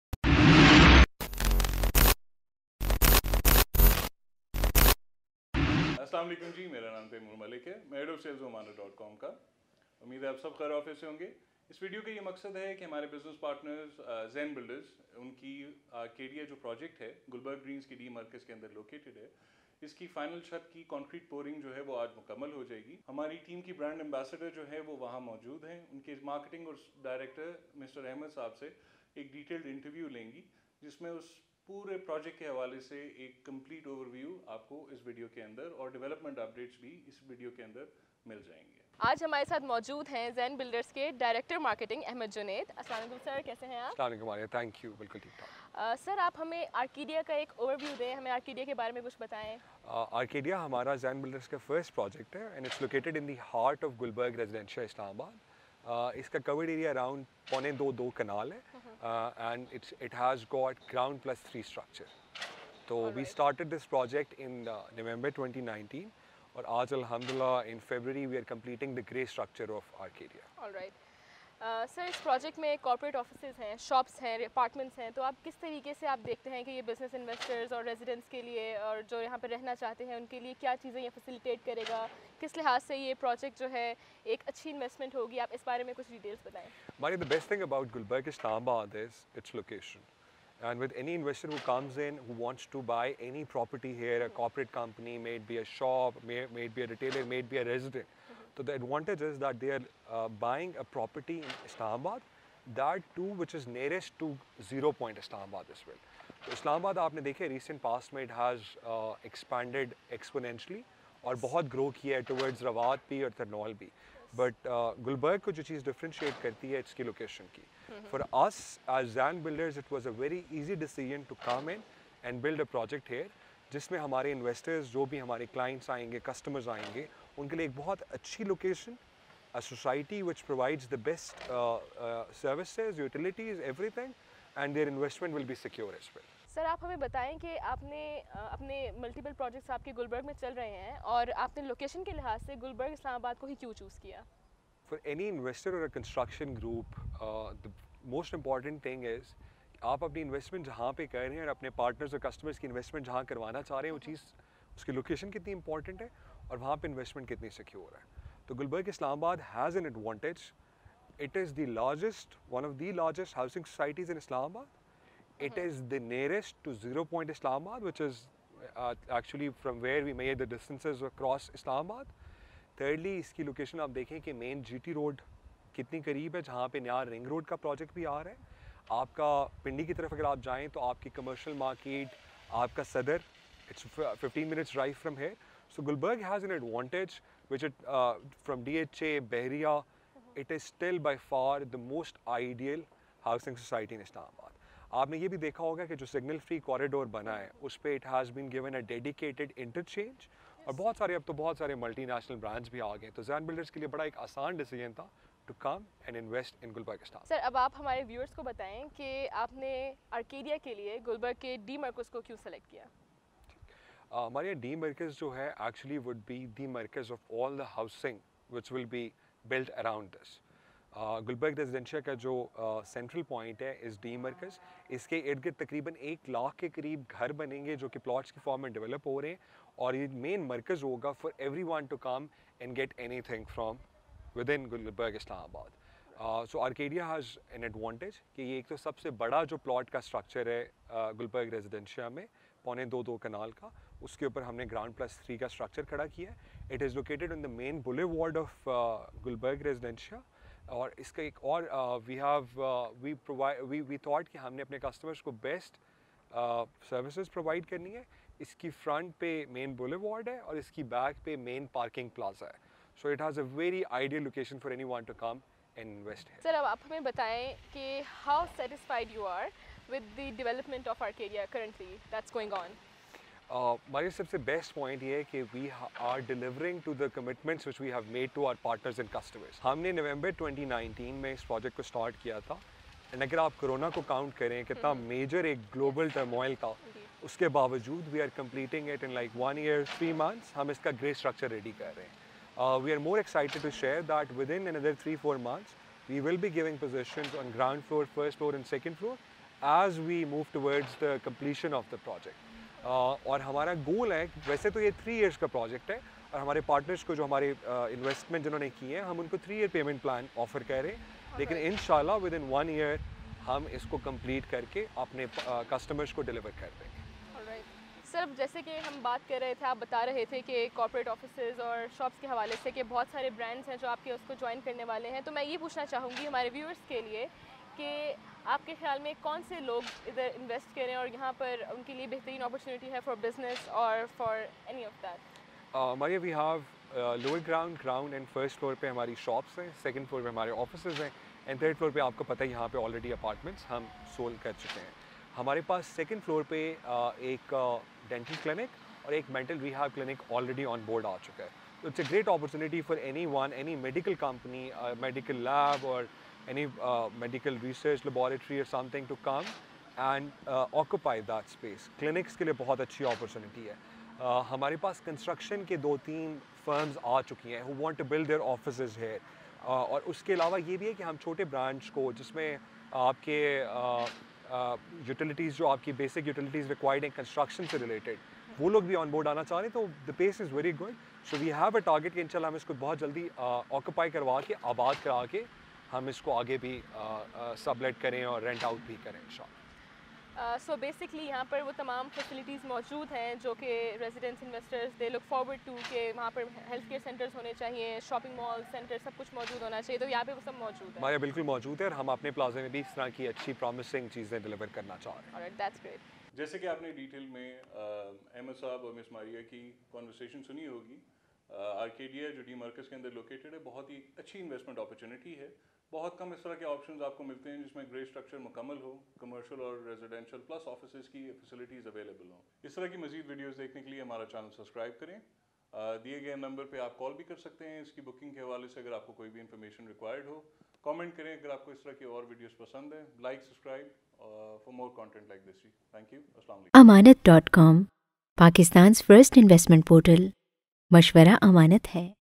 The Arcadia Latest Updates and Overview| Gulberg Greens| Gulberg Residencia Islamabad |Ammanat.com
The Arcadia is a project of Zain Builders. This multi-purpose building gives you the international standard of facilities and amenities under a single roof. The exterior of the edifice is inspired by the Victorian Architectural theme with a contemporary touch. So, get ready to upgrade your lifestyle with a complete package in The Arcadia.